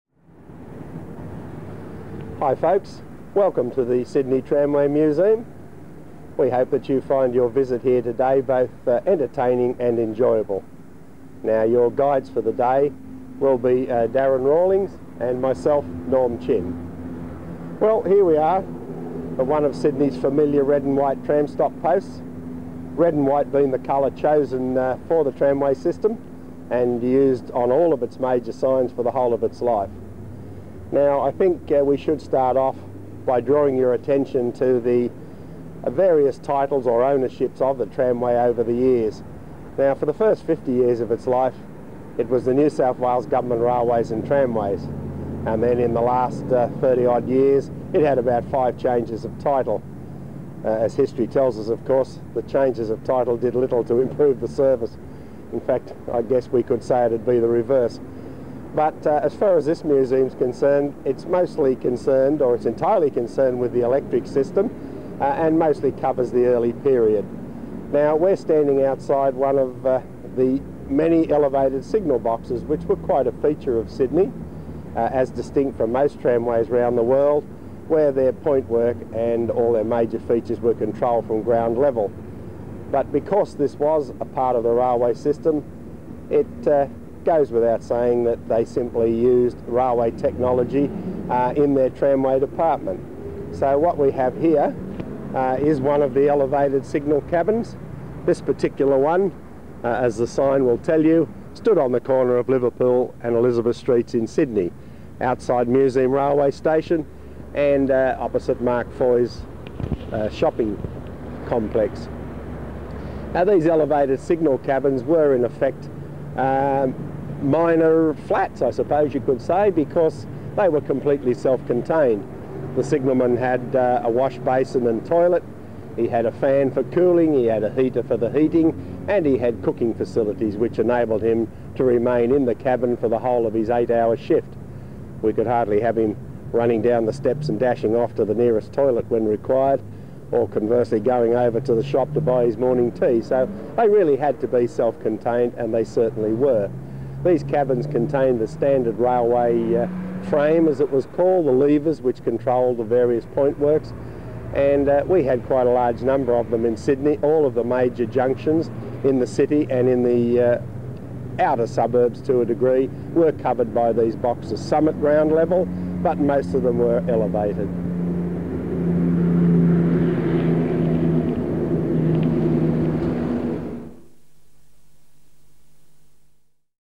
Norm Chinn Gives a Tour (1991) - Liverpool St Signal Box
From the Sydney Tramway Museum's archives. Featuring the late Norm Chinn, assisted by Darren Rawlings, giving a video tour of the museum in late 1991.

We are presenting the original VHS tape as individual segments; this first video focuses on the Liverpool Street Signal Box.